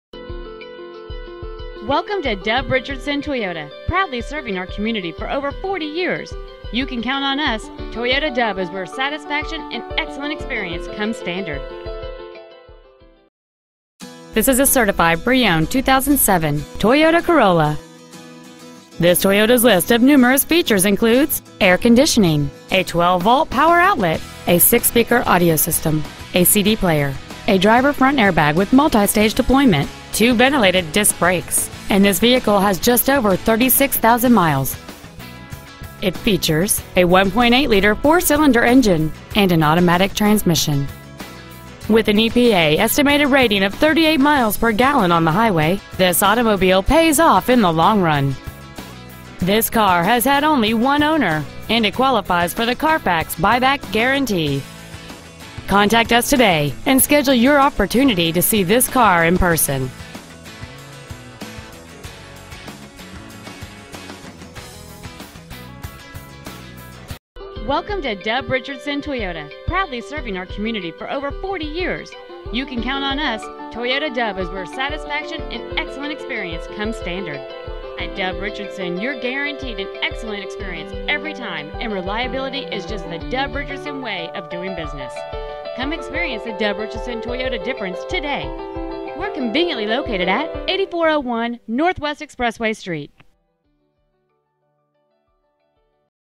2007 Toyota Corolla Oklahoma City OK
Year: 2007
 Make: Toyota
 Model: Corolla
 Engine: 1.8L I-4cyl
 Trans.: Automatic
 Exterior: Beige
 Miles: 36,881
 Interior: Beige

 Pre-Owned 2007 Toyota Corolla Oklahoma City OK